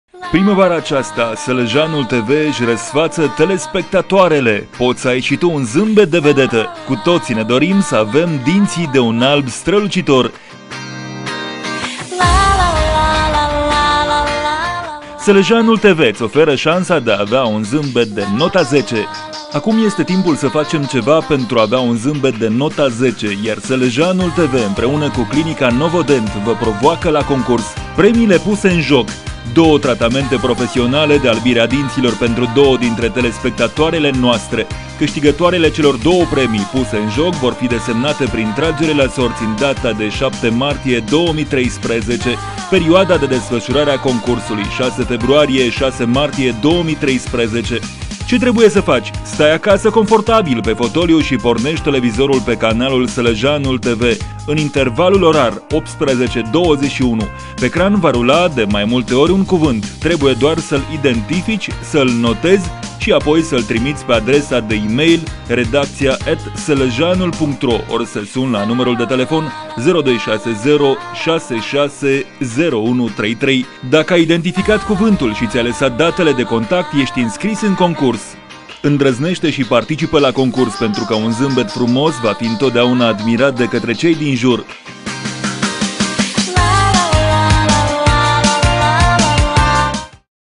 Un zambet de nota 10
Acum este timpul să facem ceva pentru a avea un zâmbet de nota 10, iar Sălăjeanul TV, împreună cu Clinica Novodent vă provoacă la concurs.